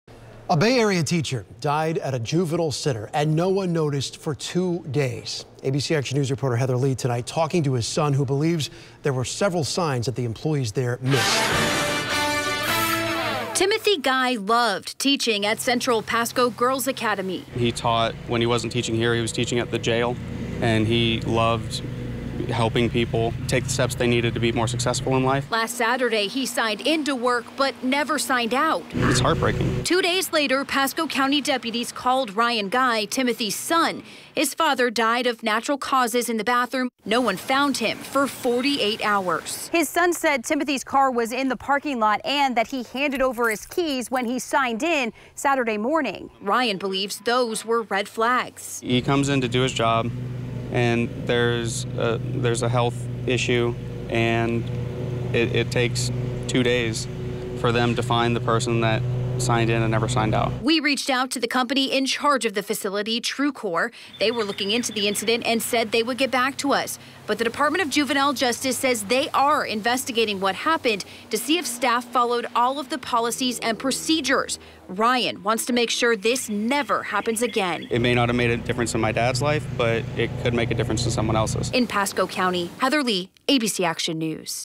No one noticed man died for two days
A Bay Area teacher died at a juvenile detention center where he works.